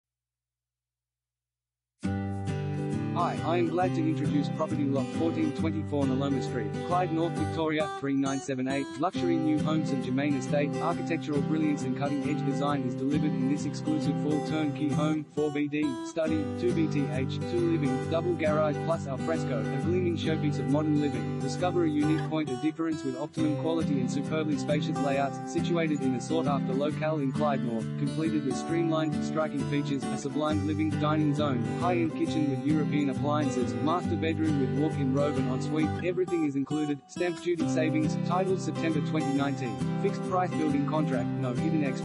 Lot 1424 Niloma Street, Clyde North VIC 3978 | AuPropertyReport.Com
Lot 1424 Niloma Street, Clyde North VIC 3978
Property Type: house
Bedrooms: 4
Bathrooms: 4
Car Space: 2
Luxury New Home St Germain Estate